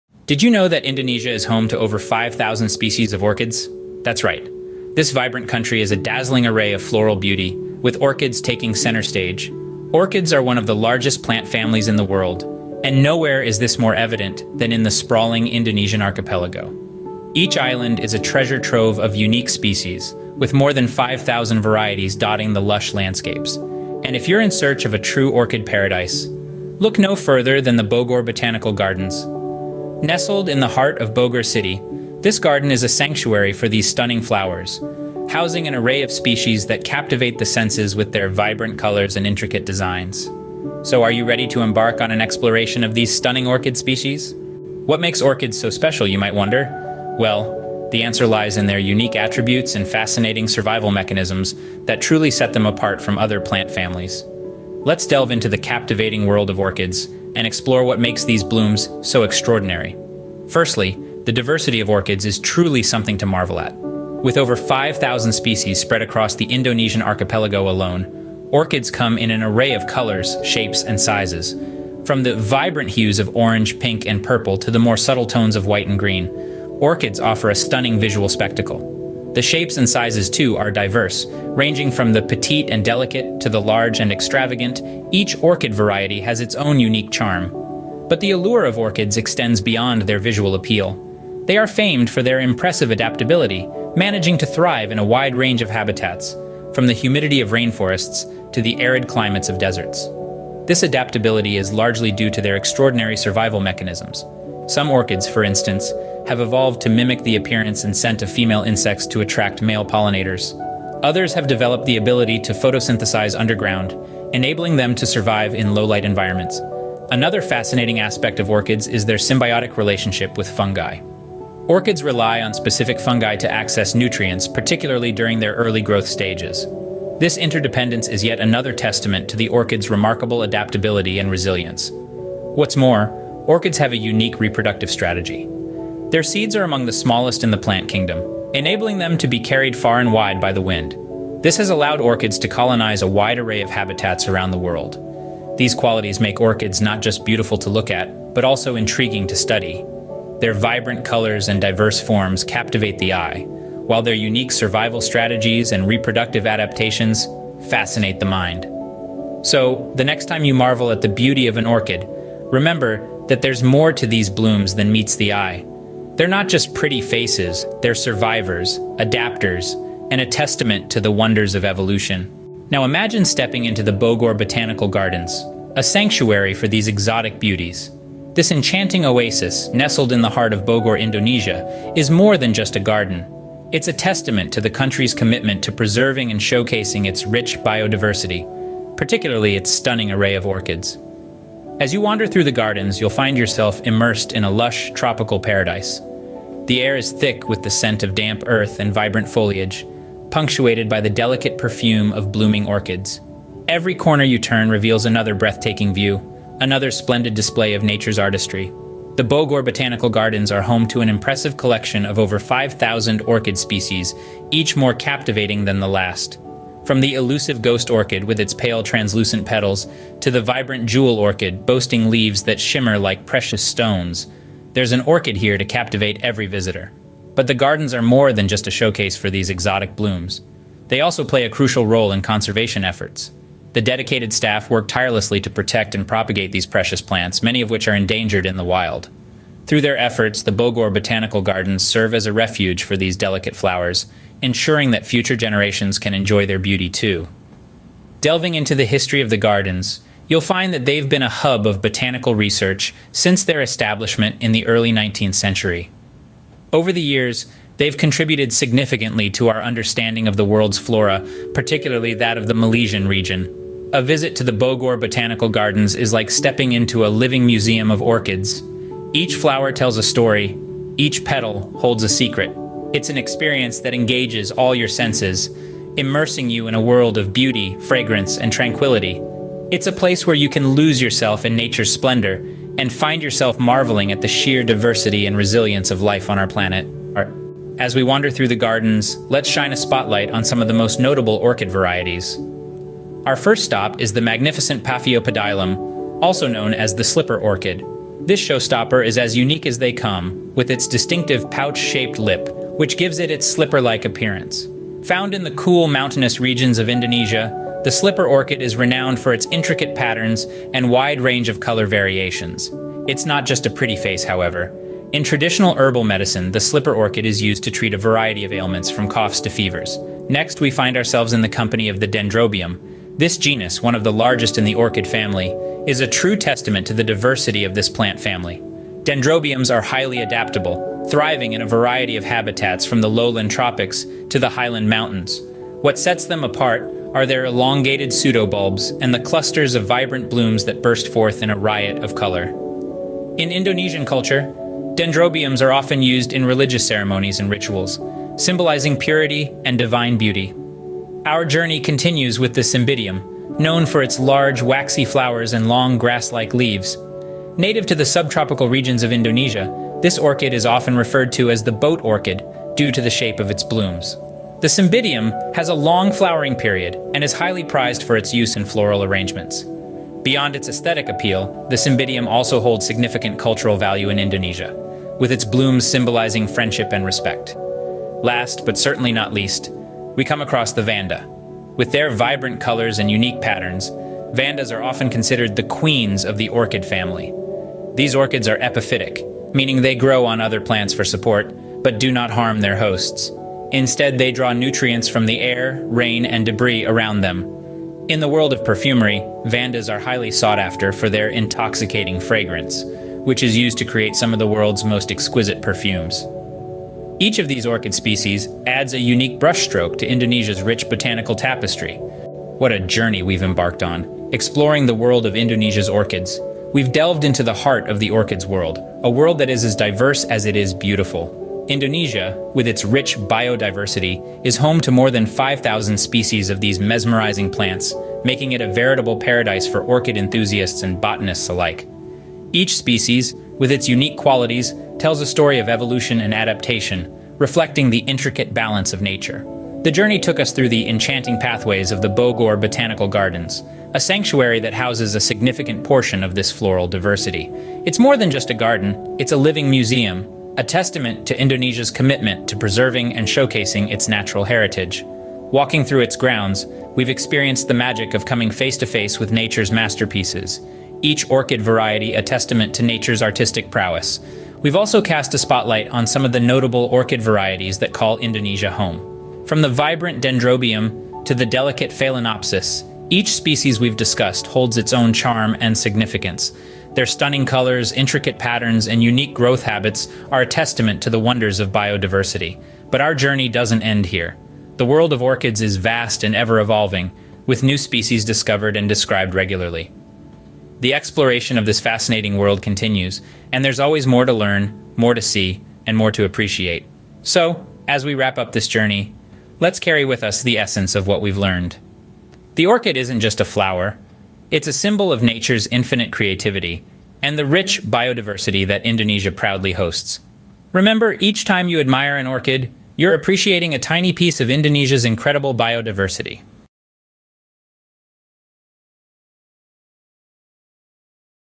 Orchid Odyssey: Indonesia's Floral Paradise
In this captivating video, we take you on an awe-inspiring journey through the stunning Bogor Botanical Gardens in Indonesia, as we explore the incredible world of orchids. Join us as we delve into the fascinating facts and figures surrounding these beautiful blooms. Whether you're a nature enthusiast or simply appreciate the unparalleled beauty of orchids, this video is sure to leave you in absolute awe. So sit back, relax, and get ready to be immersed in the mesmerizing array of orchids found in the Bogor Botanical Gardens.

OUTLINE:

00:00:00 Introduction to Indonesia's Orchid Diversity
00:00:44 The Unique Qualities of Orchids
00:02:52 Experiencing the Bogor Botanical Gardens
00:04:47 Spotlight on Notable Orchid Varieties
00:06:52 Recap and Conclusion